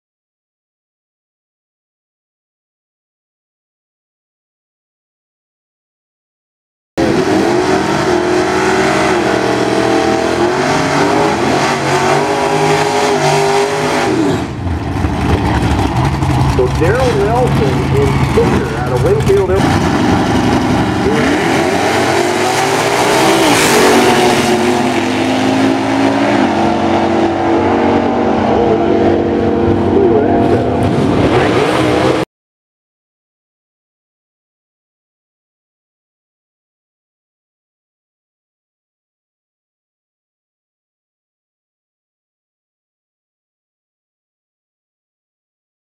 Classic Ford vs Chevy matchup at the 2023 Time Machine Nationals
BCG Erick Esse in his 56 Chevy
vs
BCG Daryl Nelson in his 65 Merc Comet